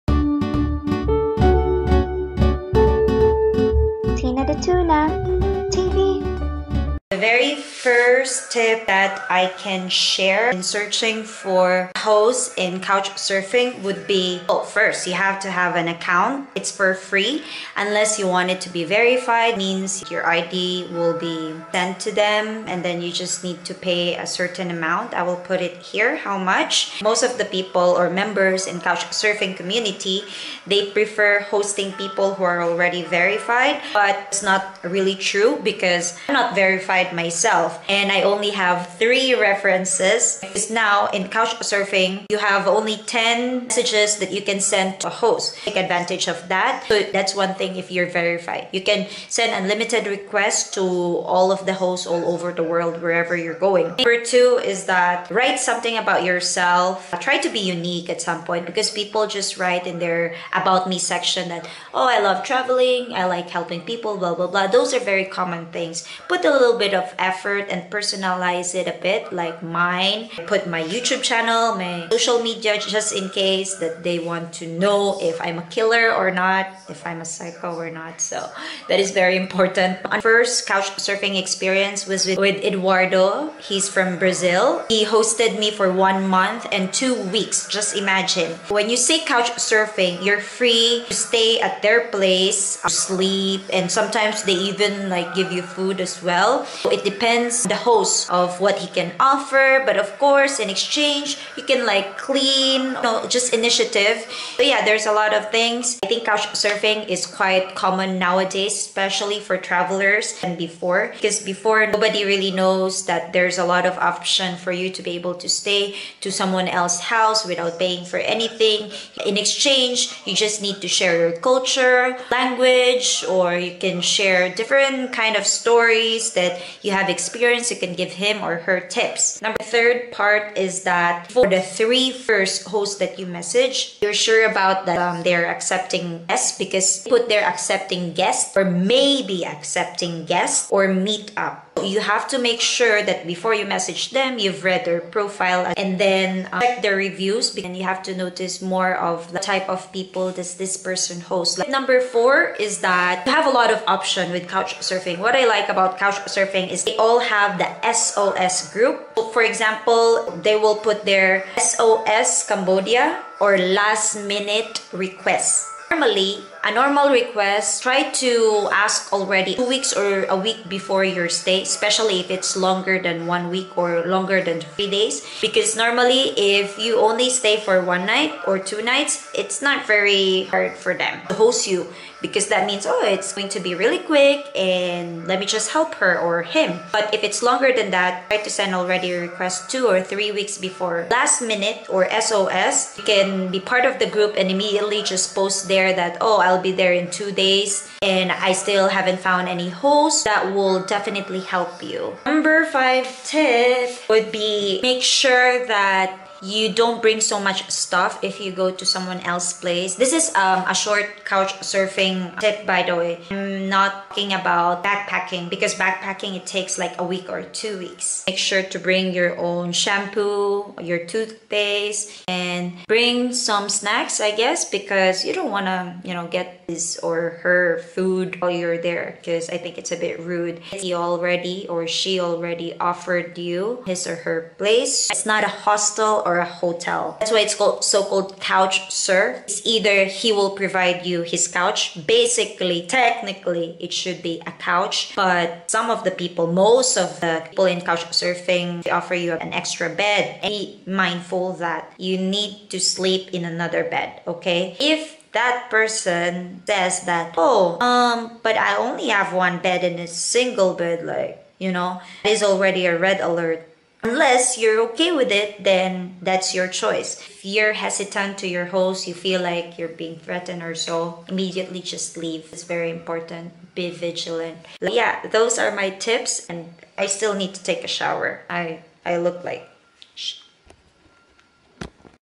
VLOG #21: How To Do Couchsurfing - Tips you need to know!
If you're watching this video that means you are ready to travel for FREE!!
To meet new people, discover new places and share your travel journeys with the rest of the world!
Indeed you're born a traveller! I hope this tips can help you start your first Couchsurfing adventure, I wish I knew!

If you need more tips, do write down in the comments section :)
Enjoy and happy travels!

(Literally I just woke up look, lol)